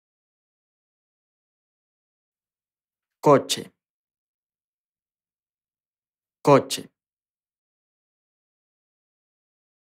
How to pronounce COCHE in Latin American Spanish
This video shows you how to pronounce COCHE in Latin American Spanish. Speaker has an accent from Queretaro (central Mexico) . Collins Dictionary, the home of living language and pioneers of dictionary publishing